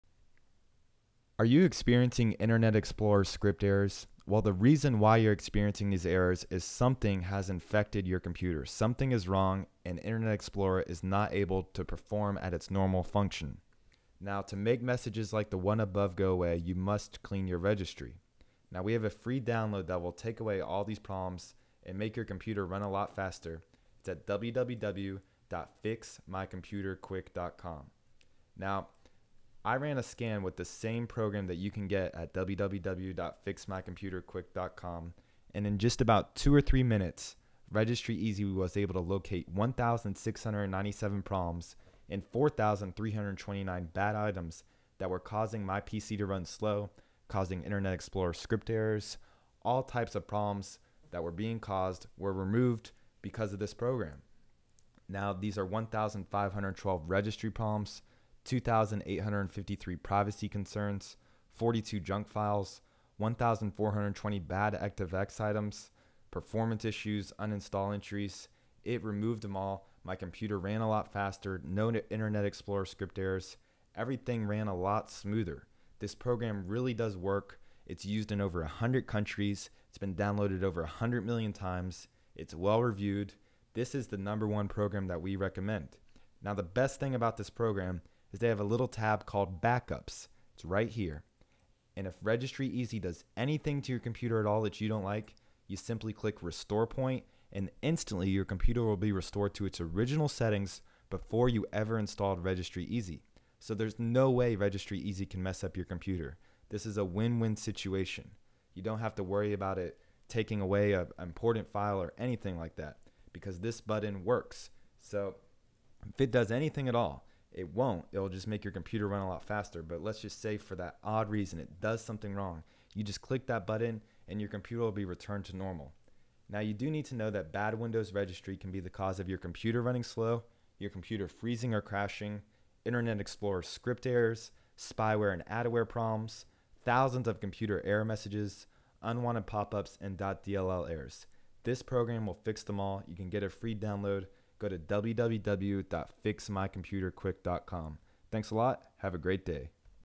Internet Explorer Script Error Problems? Free Download Fix
- Free Download To Fix Your Internet Explorer Script Problems

In this video you will learn why your experiencing internet explorer script problem as well as how to fix them!

Get the Free Download To Fix Your Internet Explorer Script Problems